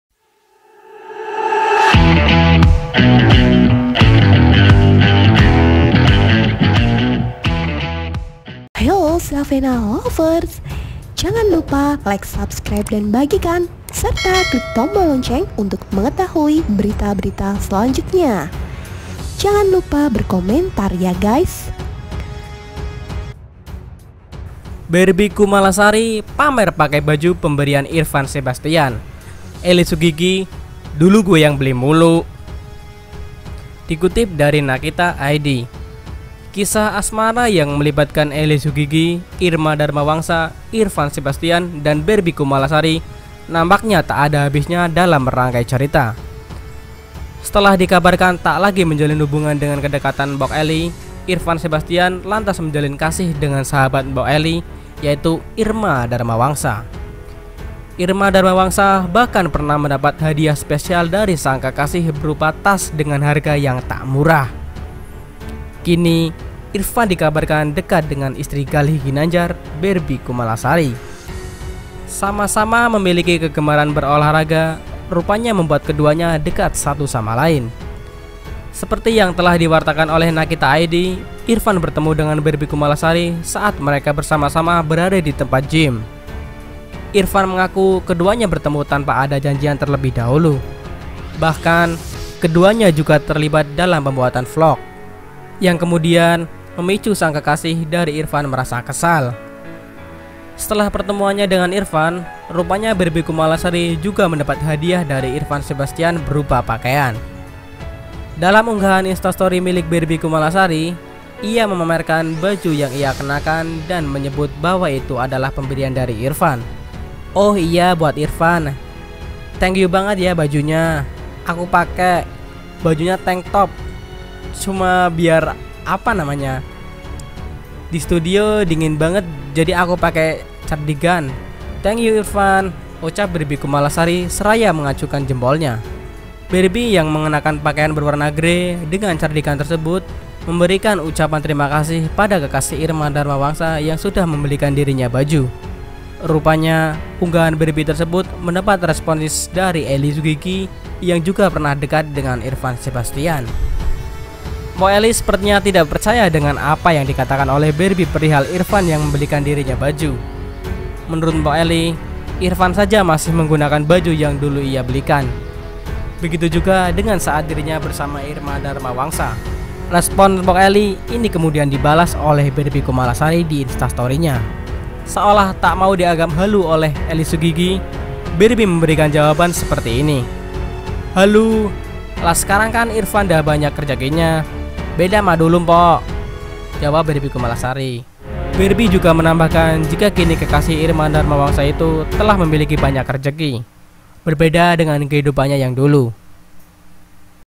Barby Kumalasari dikabarkan dekat dengan Irfan Sebastian , Begini Tanggapan sang Mantan (Ely Sugigi)
BarbyKumalasari IrfanSebastian EliSugigi Terima kasih sudah berkunjung ke Channel Zavena, Portal gosip yang paling up to date.